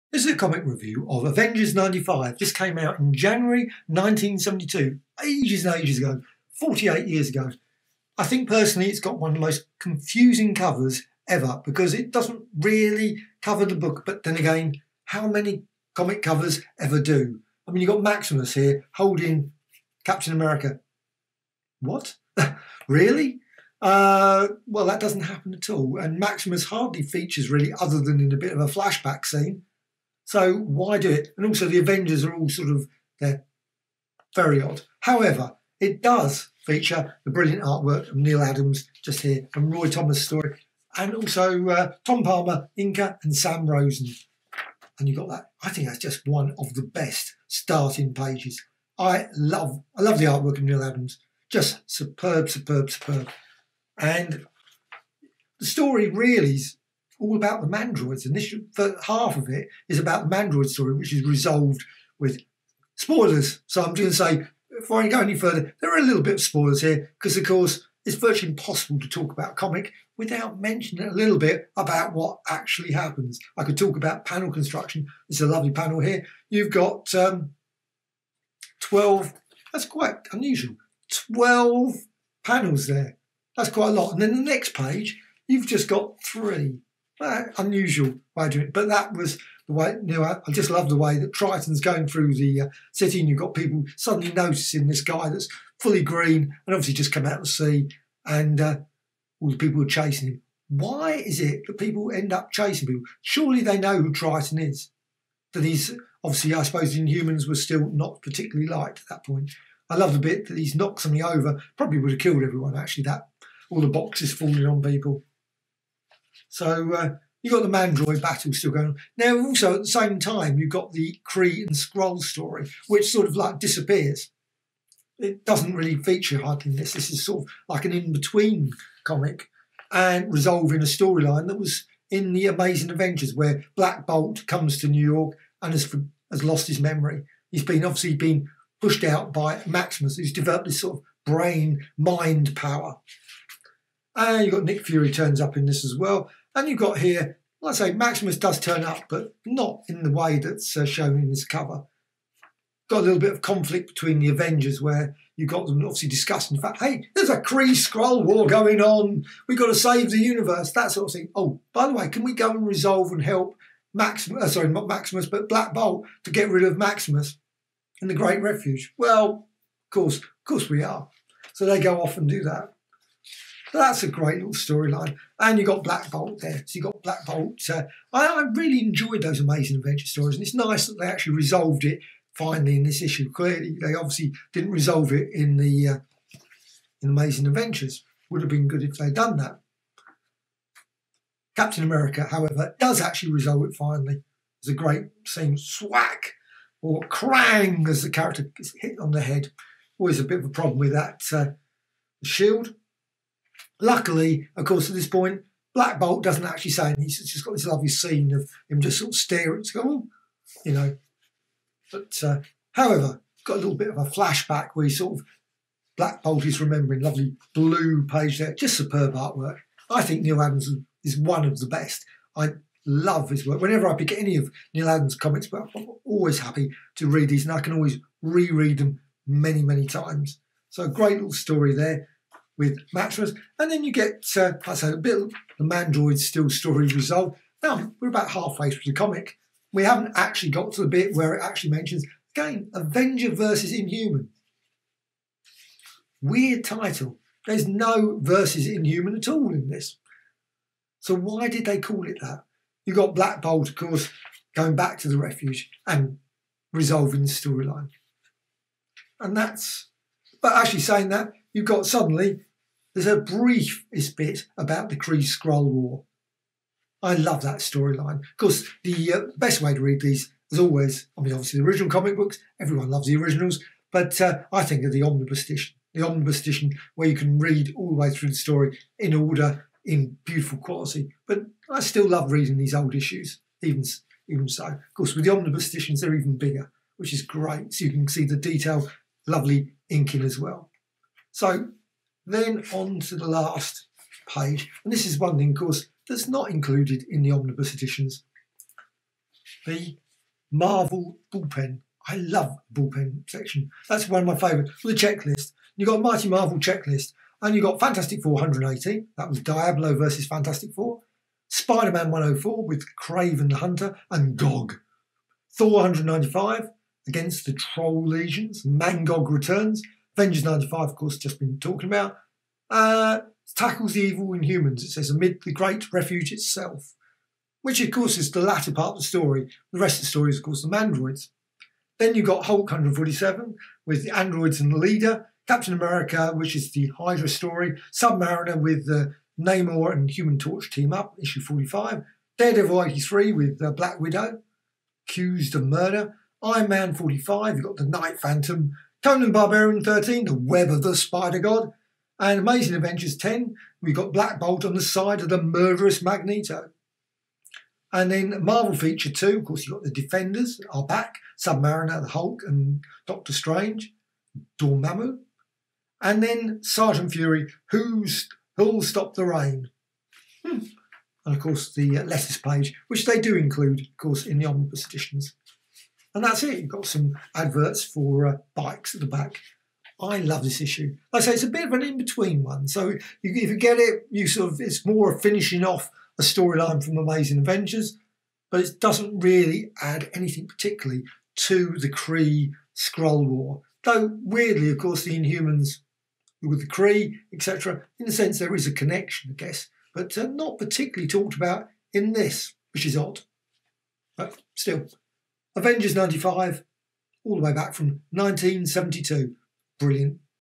"Avengers 95" January 1972 from Marvel (Roy Thomas Neal Adams) Inhumans Book Review
Avengers 95 --- January 1972 from Marvel (Roy Thomas Neal Adams) Inhumans Book Review - an in-between / tidy-up (from Amazing Adventures) story with the Inhumans (Triton, Maximus, Black Bolt etc) and half of the Avengers with a reference to the Kree-Skrull wars